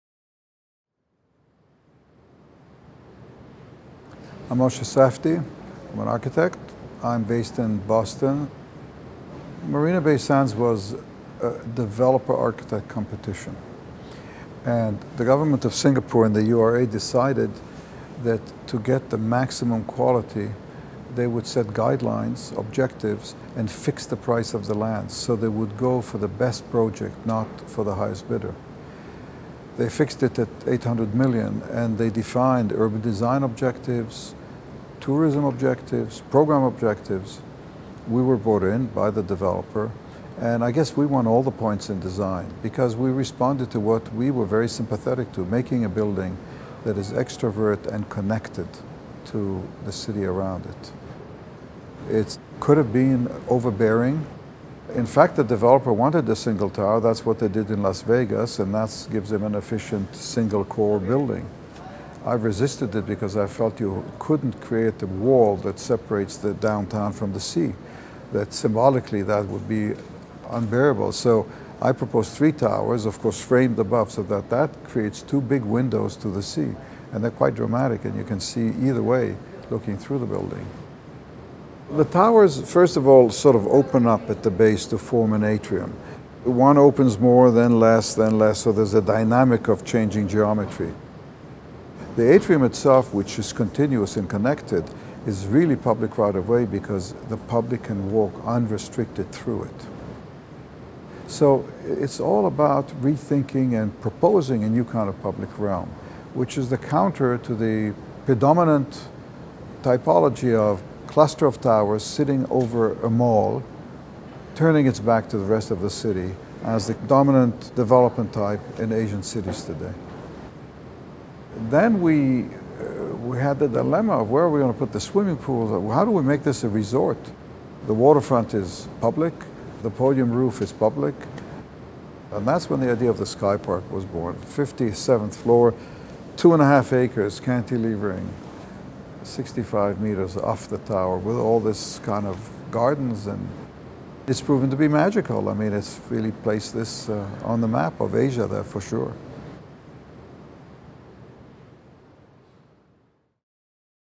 Moshe Safdie interview: Marina Bay Sands in Singapore | Architecture | Dezeen
This movie featuring Moshe Safdie of Safdie Architects speaks to Dezeen about the Marina Bay Sands development in Singapore.